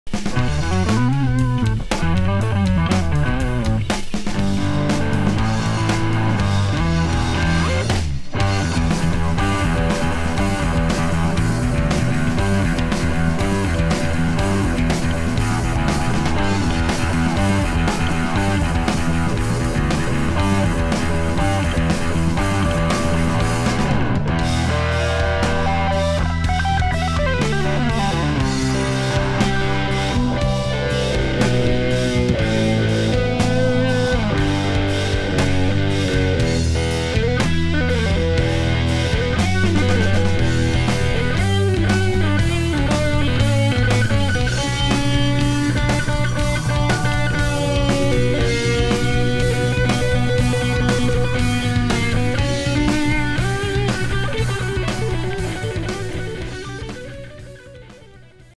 Cort G280 Select Playthrough with George Pennington | Velvet Below
Watch as shred master George Pennington III takes you through the tones on the Cort G280 Select.
The G280 Select is a revamped follow up of the previously popular G280DX, with newly added pearloid pickguard, Rosewood fingerboard, and Voiced Tone VTH-77 hot humbucker with metal cover for those boutique high-grade looks and solid build quality. Designed with the best possible playability and sonic performance in mind, the G280 Select comprises an Alder body with Flame Maple top and HSS pickup configurations for a versatile sound that can be used for both classic and modern musical genres.